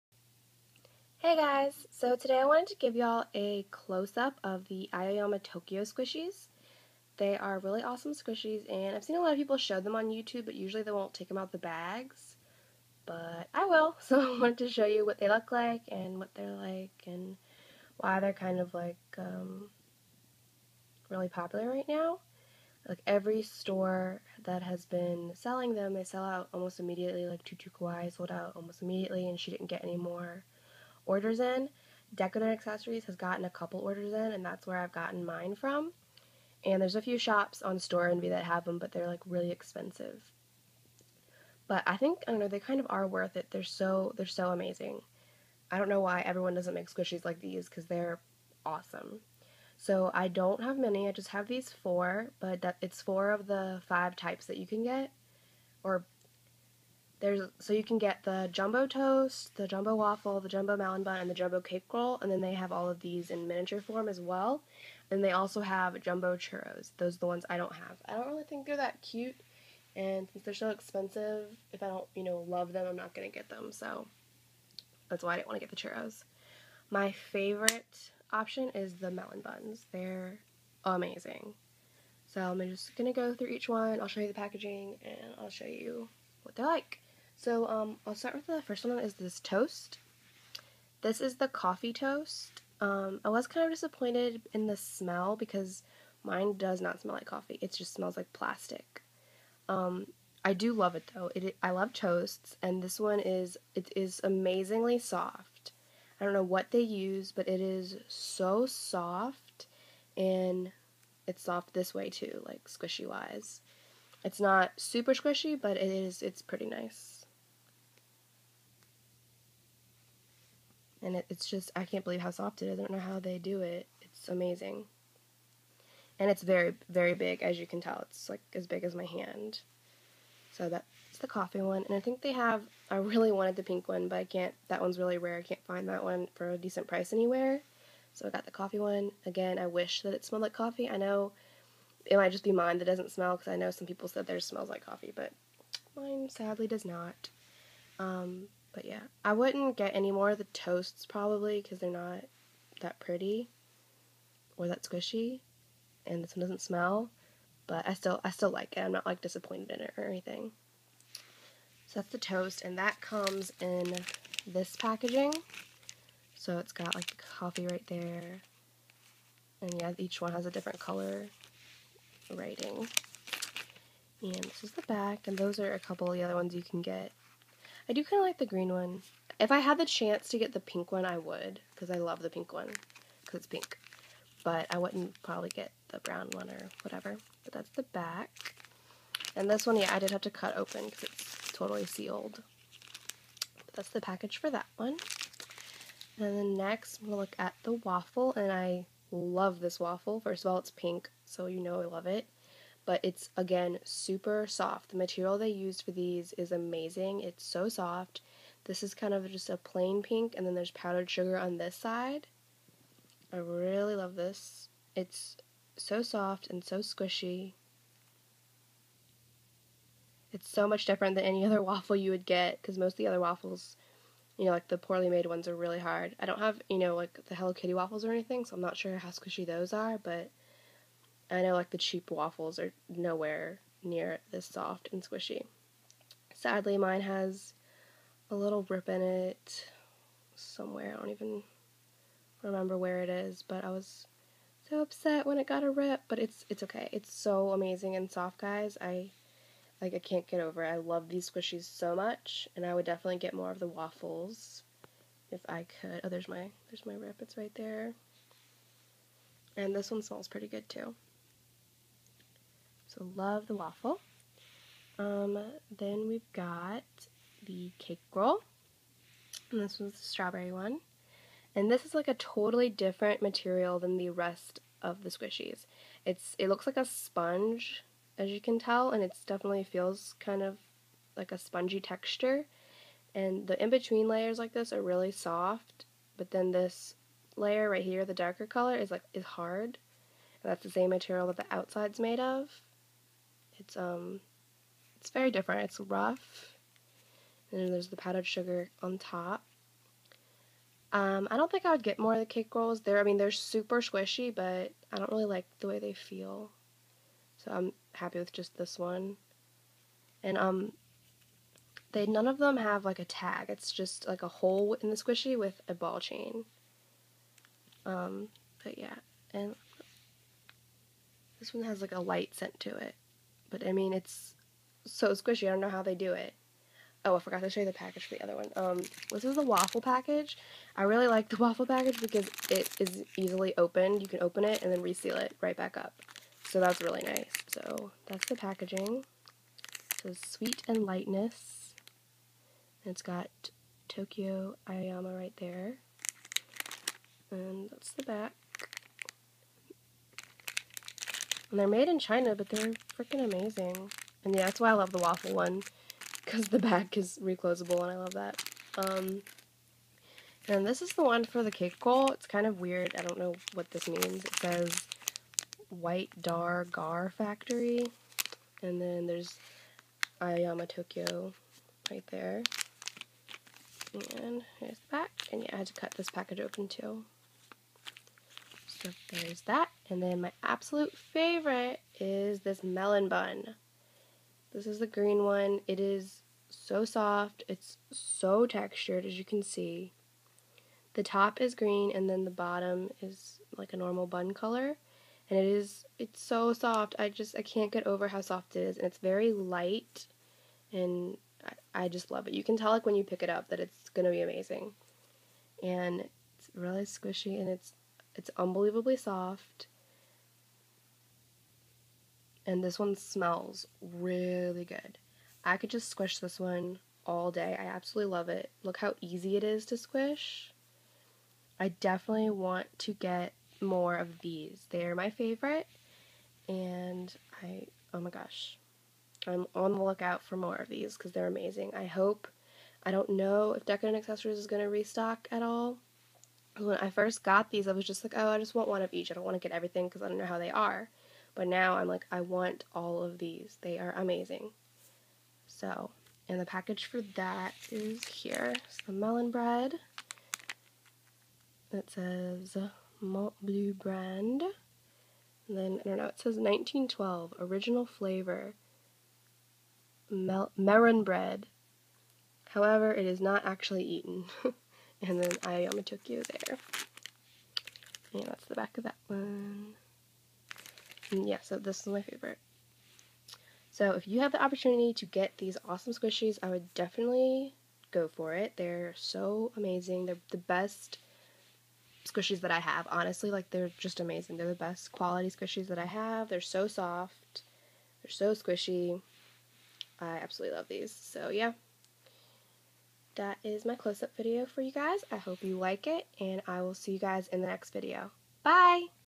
Aoyama Tokyo Squishy Closeup (from It's Cute)
Hey y'all here is a closer look at the awesome Aoyama Tokyo Squishies.  I got mine at Decoden-acc.com and there are still a few left!